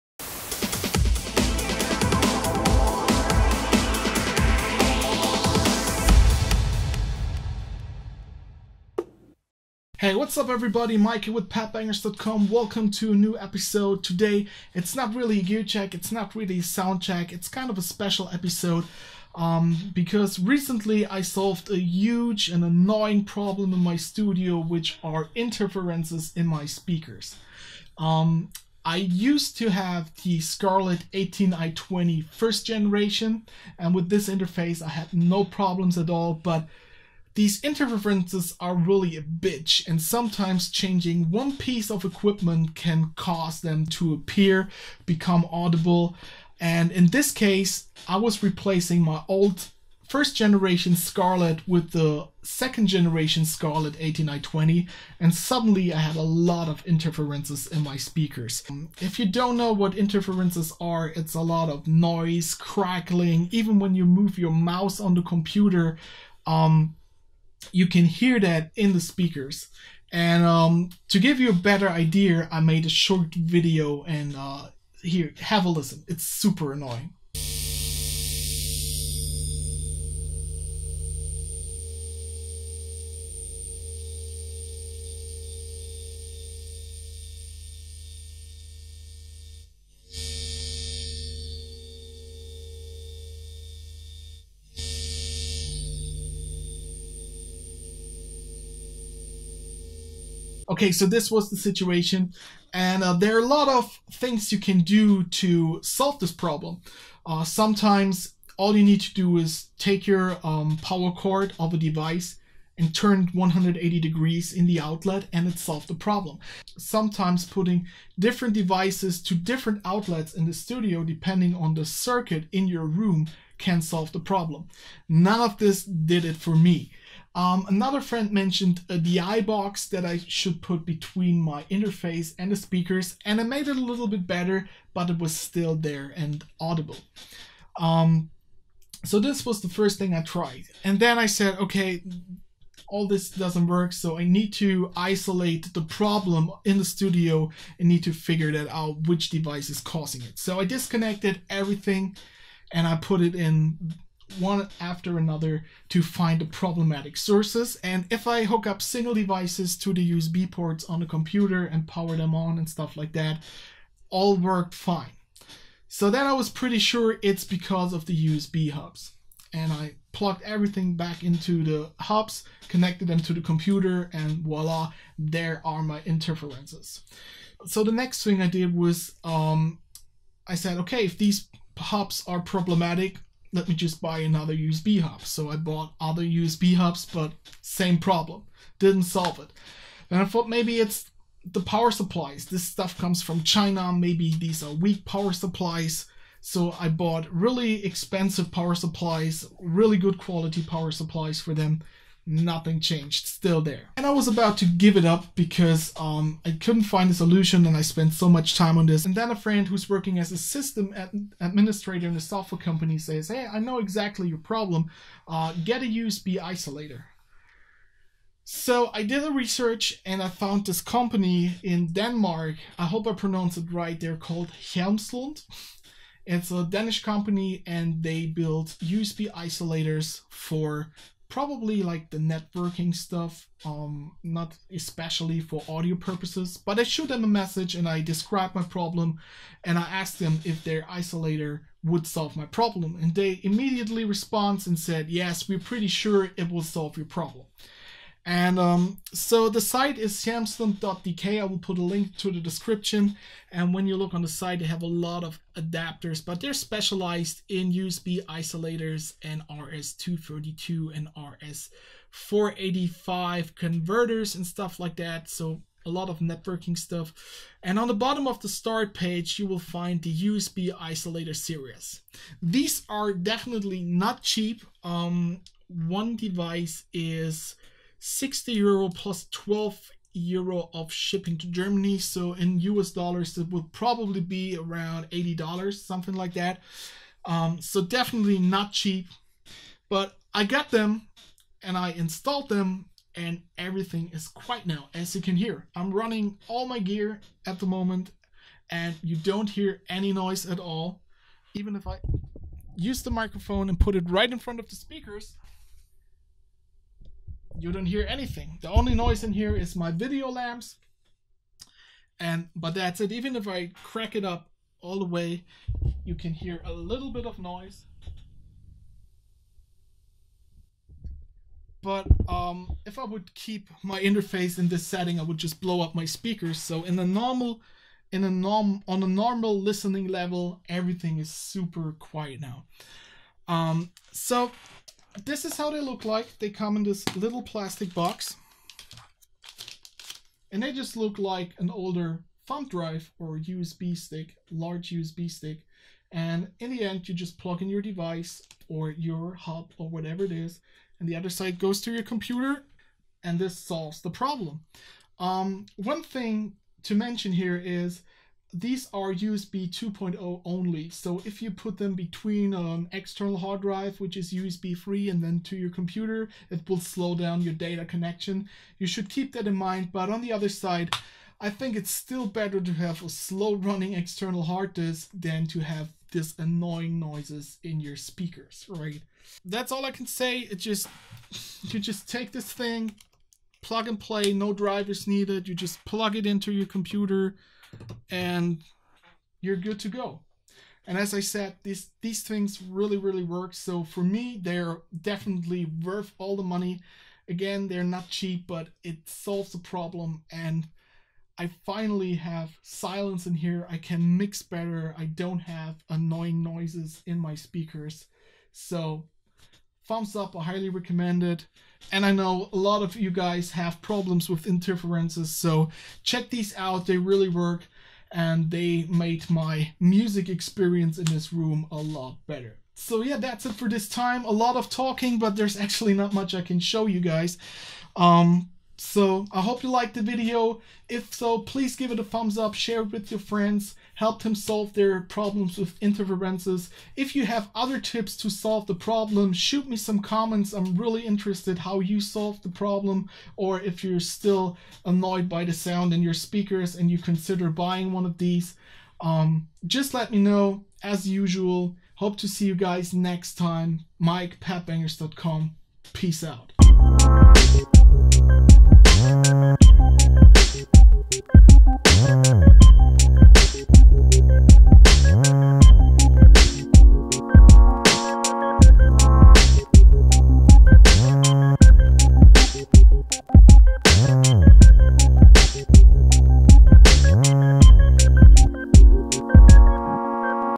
Hjelmslund USB Isolators Review Shut up Noise in your speakers
In this episode we talk about USB Interferences and how to stop the annoying noises in your speakers. After hours of investigating the problem and trying different approaches to solve it, we ended up with USB Isolators sold by Hjelmslund in Denkmark.
This little device did the trick. Check the video for all the details.